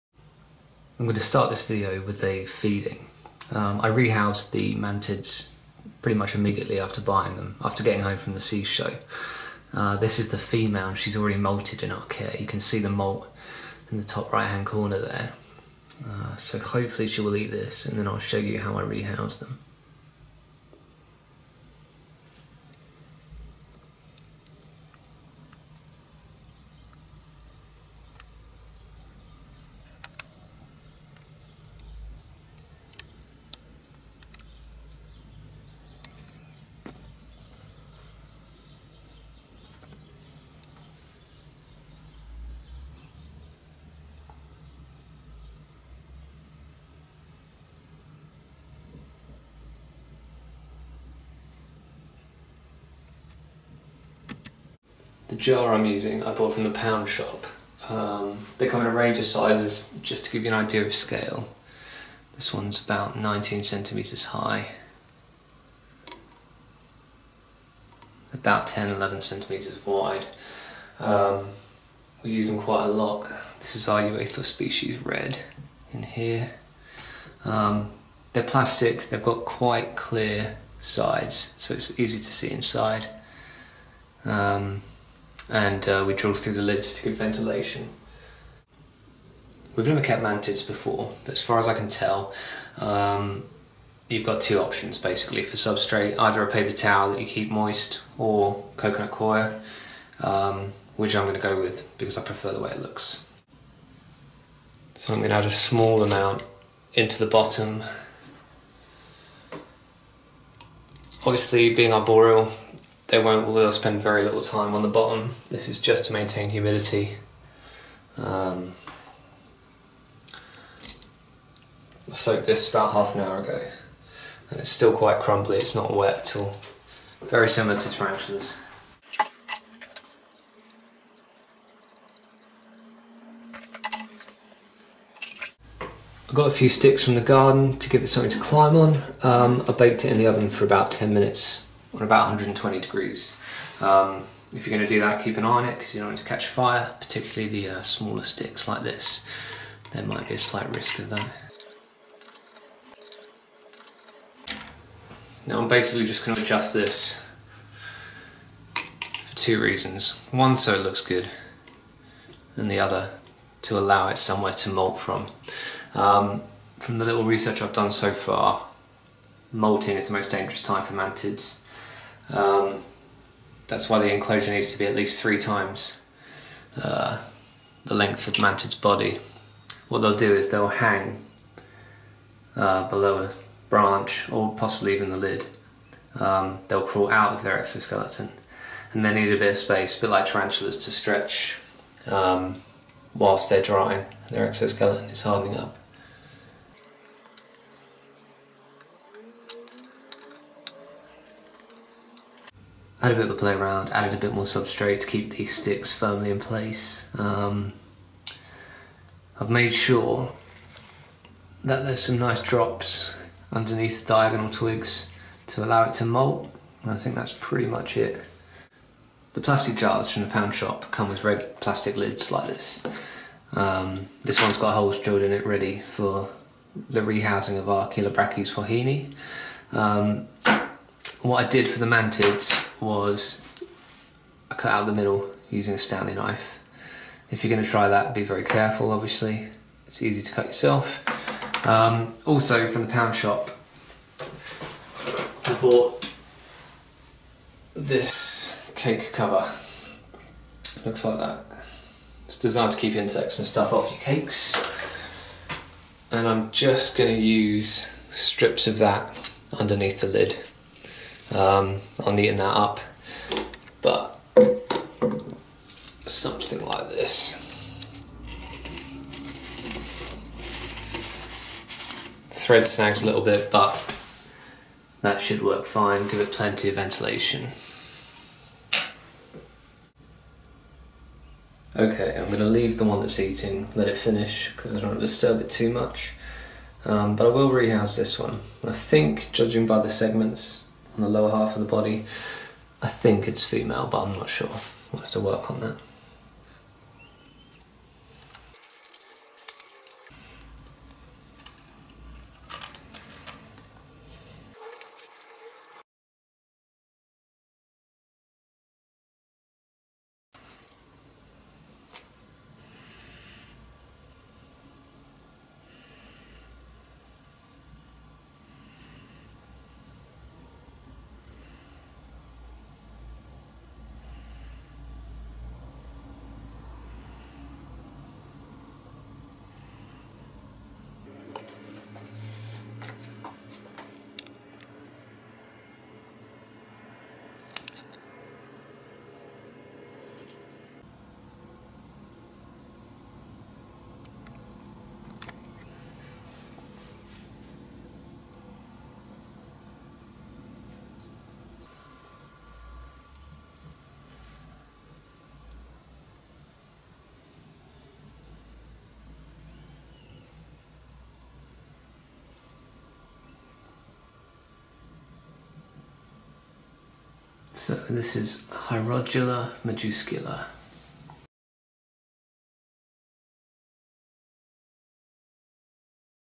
Setting up and housing Hierodula Majuscula (Giant Rain Forest Praying Mantis)
We picked up two giant rain forest mantids from Metamorphosis whilst at the SEAS show. This is the first time we've cared for this specie so please comment if you can offer any advice.

They are fascinating ambush predators and we'd highly recomend them.